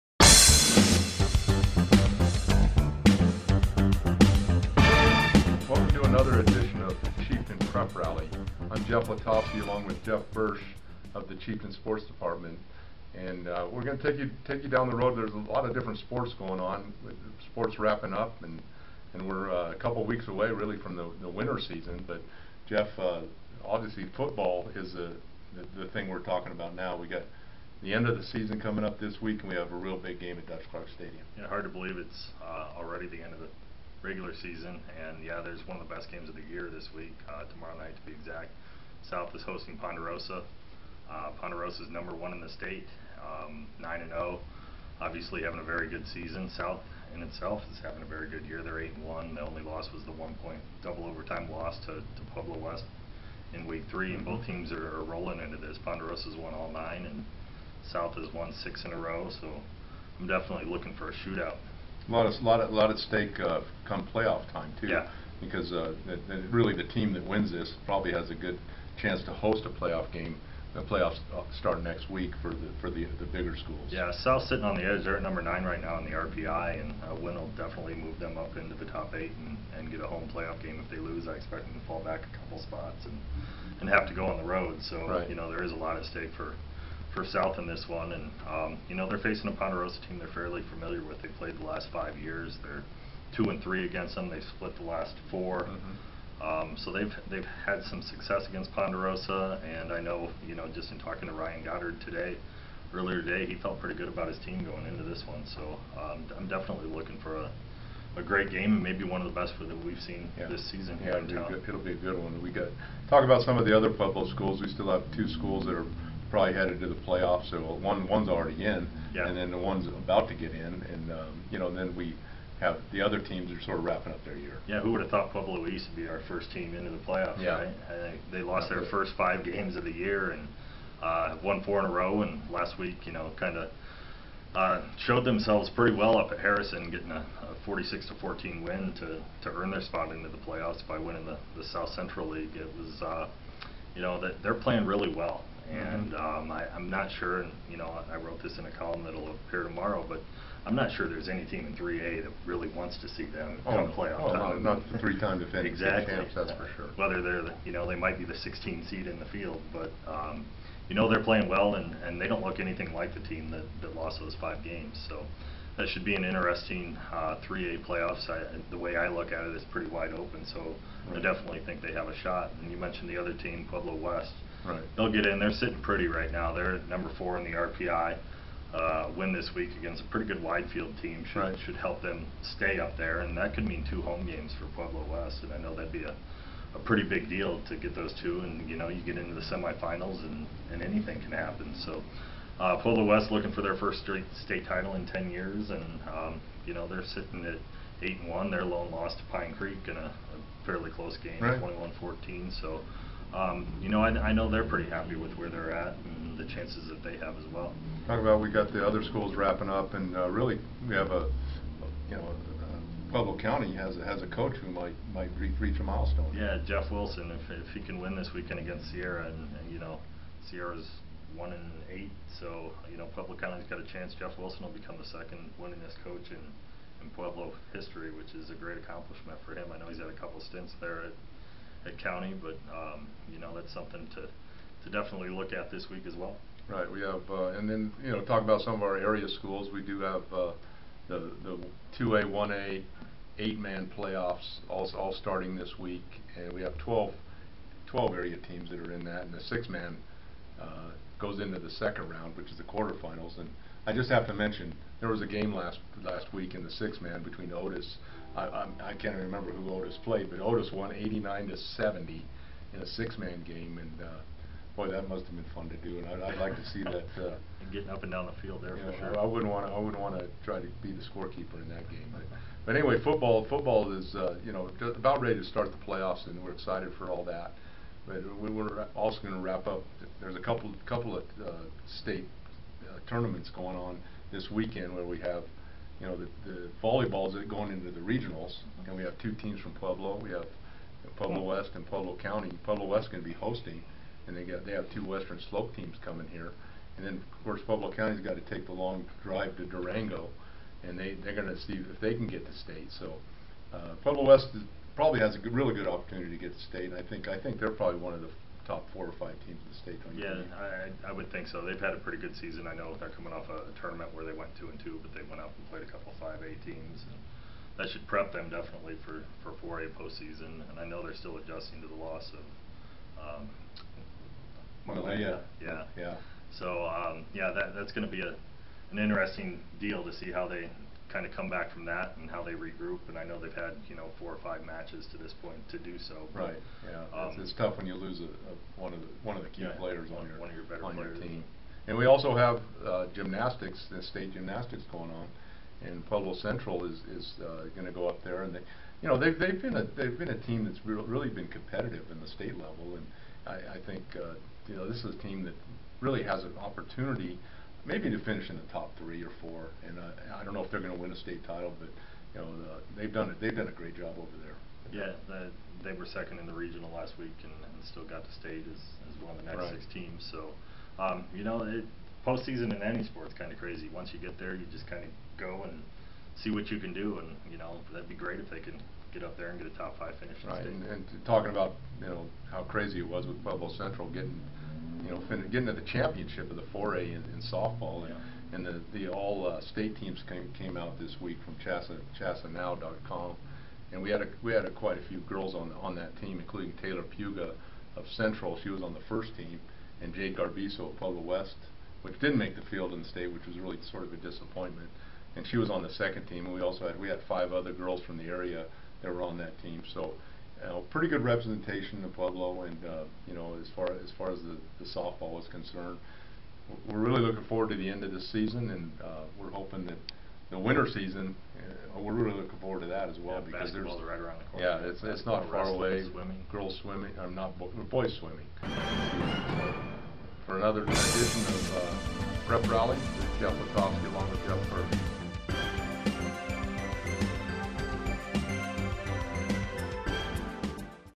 Prep Rally: Jeff squared on fall sports
Chieftain sports writers Jeff Letofsky and Jeff Bersch look ahead at the prep football season and more.